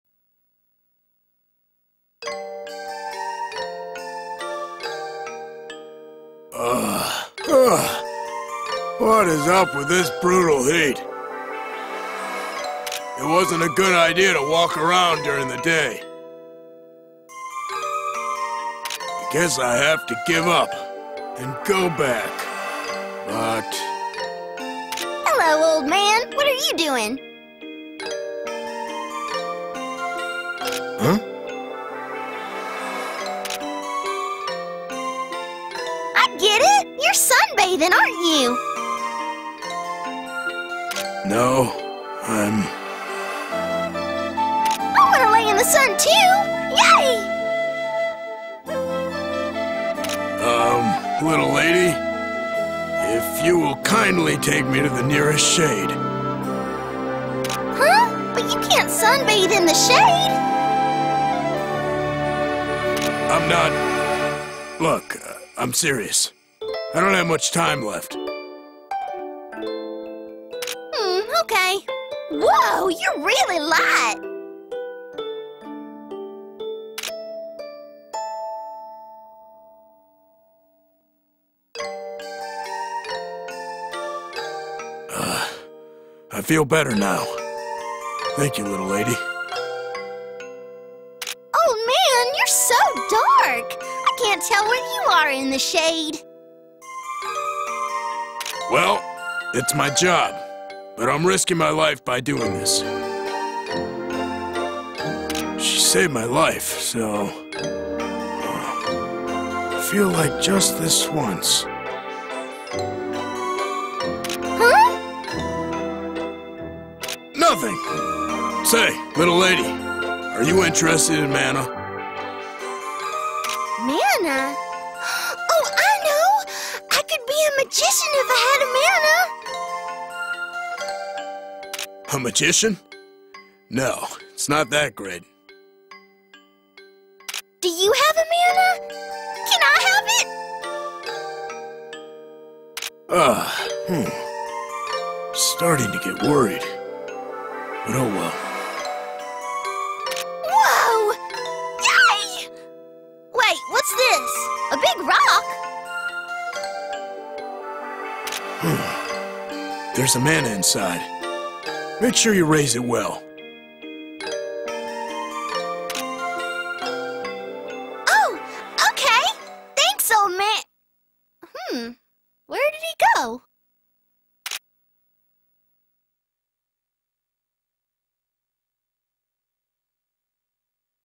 Ulrika Receives Mana Egg
A mysterious figure gives Ulrika a stone that has a Mana inside.  Ulrika wants to grow up to be an alchemist.